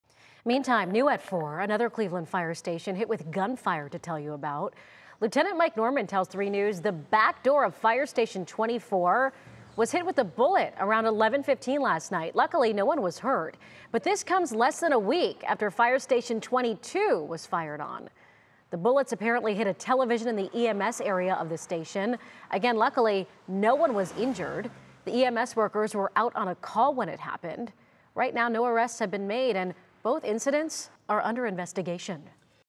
Cleveland Division of Fire confirms 2nd station struck by gunfire in less that a week
Officials say a Cleveland Division of Fire station was struck by gunfire late Sunday night, less than a week after another station was also hit by at least one bullet.

According to Lieutenant Mike Norman, the incident occurred around 11:15 p.m. Sunday at Station No. 24 on Clark Avenue, within the Detroit-Shoreway neighborhood. He told 3News a bullet struck the back door frame, then landed on the floor.

Norman said the bullet did not cause any damage. No injuries were reported.

This comes just days after firefighters from Fire Station No. 22 on St. Clair Avenue reported that multiple shots were fired at their building last week. One bullet went through a window in the EMS room and struck a television set before hitting an air duct and coming to a rest back on the window. Again, no one was hurt.